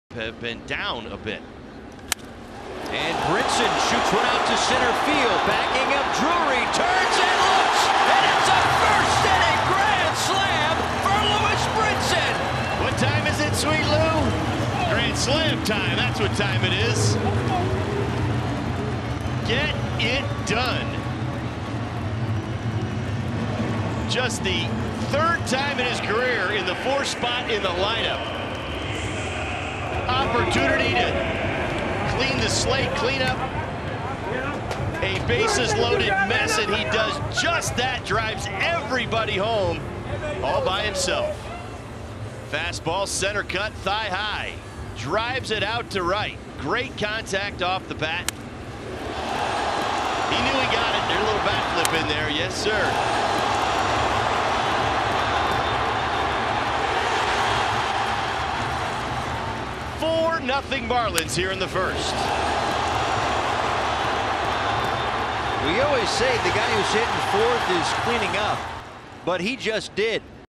Lewis Brinson Grand Slam: 8/2/2021
Lewis Brinson mashes a grand slam to right-center field to open the scoring at 4-0 in the bottom of the 1st inning.